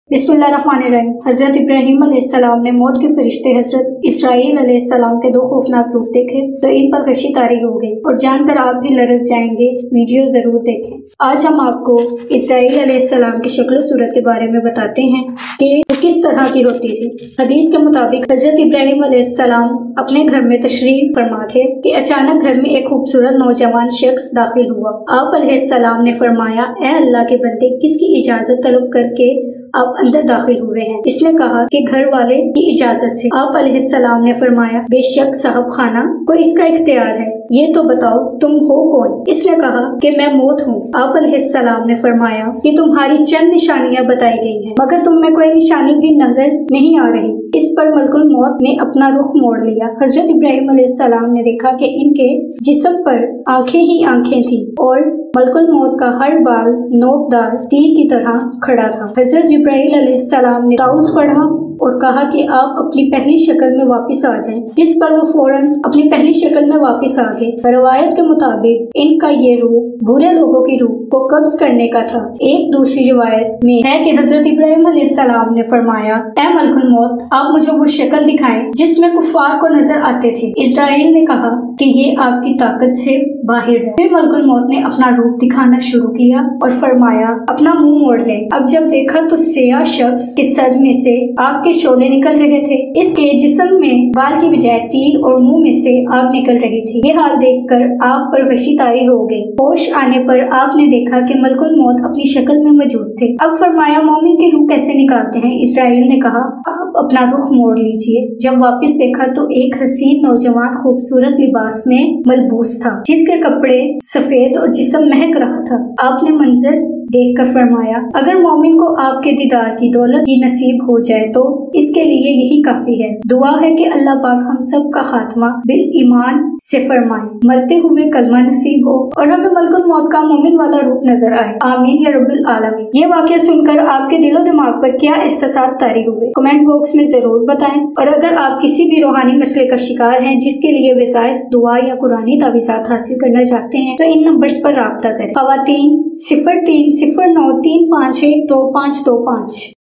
Hazrat Ibrahim Or Mout Ka Farishta | Izraeel K 2 Khoofnak Roop | Izraeel Ka Waqia
Aaj Ke video ka mozu hai.Hazrat Ibrahim Or Mout Ka Farishta | Izraeel K 2  Khoofnak Roop | Izraeel Ka Waqia. Aaj hum apko izraeel ke shakal o sorat k bare main btay gy. Hazrat ibrahim ny kya dekha mulkul mout apny bohat sary roop badlta hai . Is ke bohat sari nechaniyan hain. Ye apny bohat sary roop badlta hai. Dua hai k allah pak hum sub ka khatma iman par farmye or hamy qalma naseeb ho. Or hamy mulkul mout ka momin wala roop nazar ay.Is pore waqie ko janny k lye video ko mukaml dakhe. Or muzeed tafseelat k lye is number par rabta karen.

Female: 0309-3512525